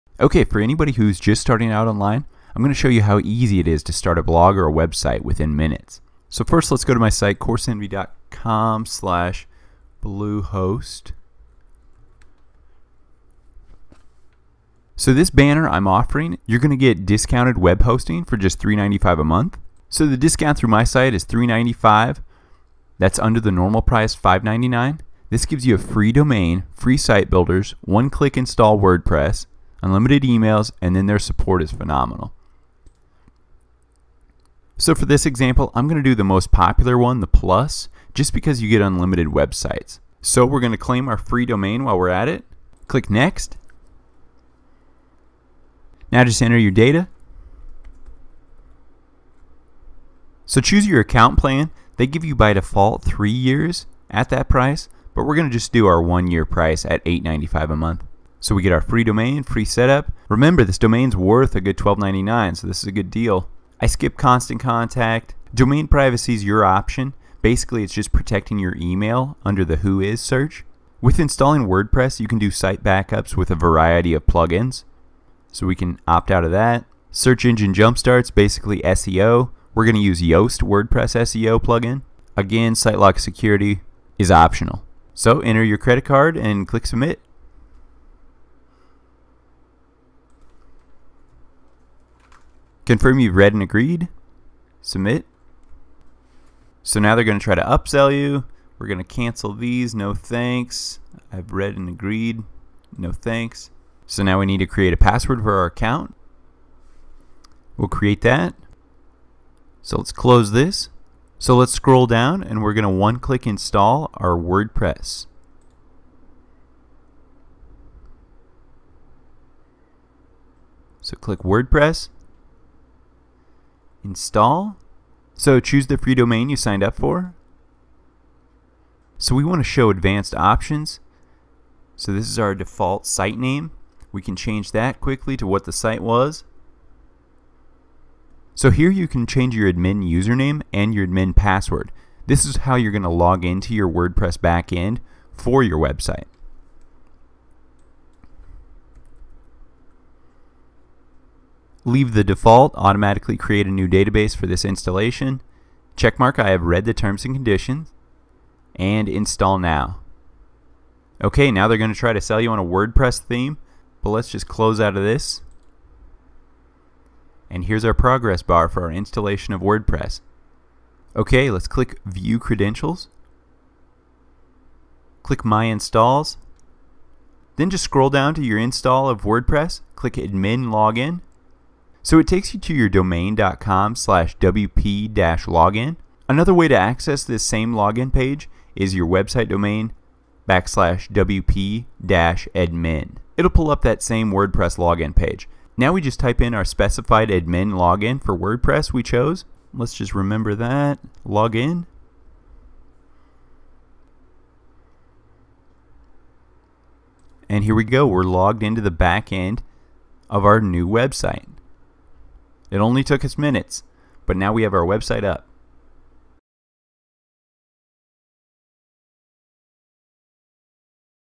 How to Build a Blog in Less than 4 Minutes with Coursenvy
(a limited $3.95/mo. discount is available at this link only!)

Today, I'll take you (rather swiftly) through how to setup your first Wordpress blog, in less than 4 minutes!

I use bluehost.com for my web-hosting and domain services, however there are many others out there with the same capabilities that you may use if you choose to do so.